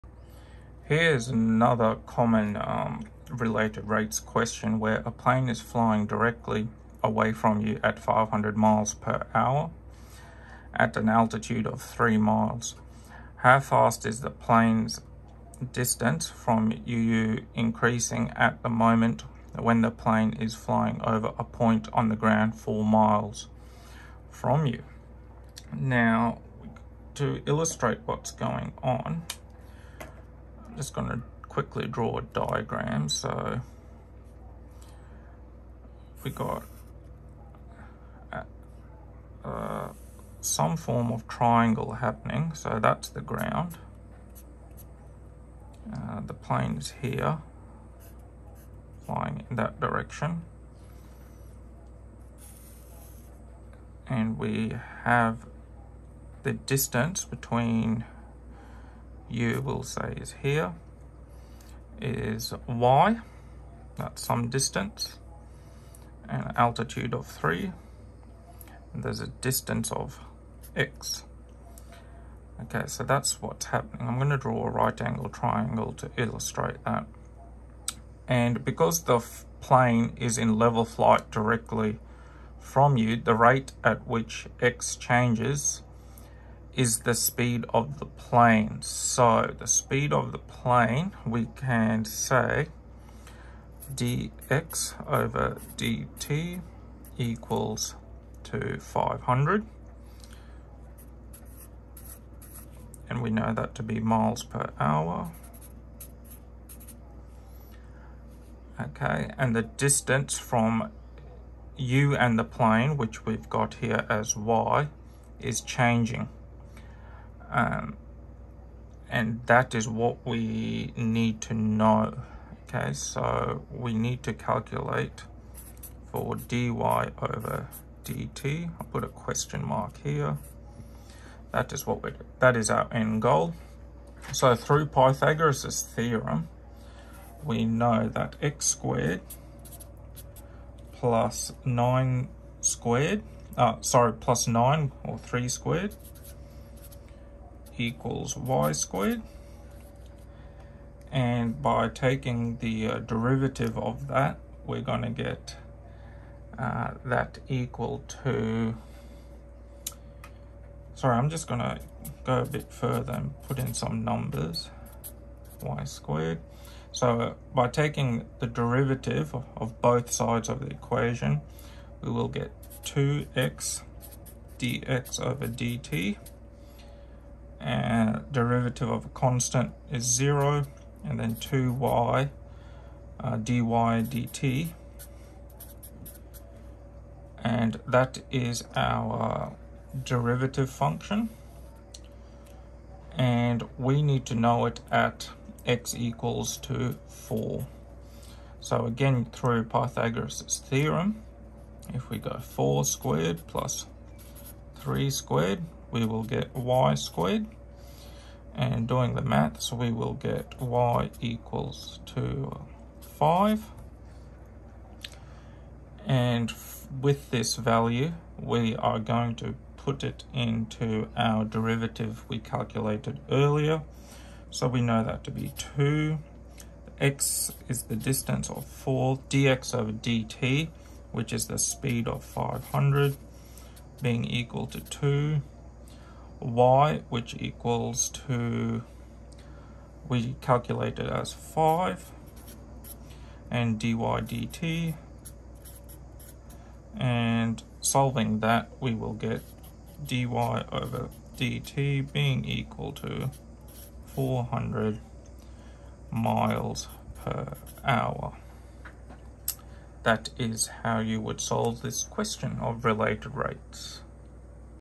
Related Rates - Plane Flying
A plane is flying directly away from you at 500 mph at an altitude of 3 miles. How fast is the plane's distance from you increasing at the moment when the plane is flying over a point on the ground 4 miles from you?